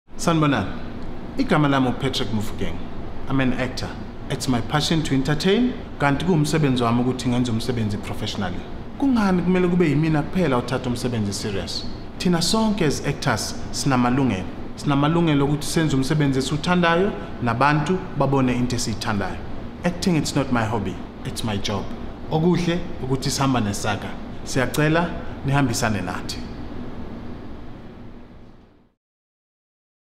Patrick Mofokeng SAGA PSA
Patrick Mofokeng explains why he feels the South African Guild of Actors (SAGA) is a worthwhile cause for South African Actors.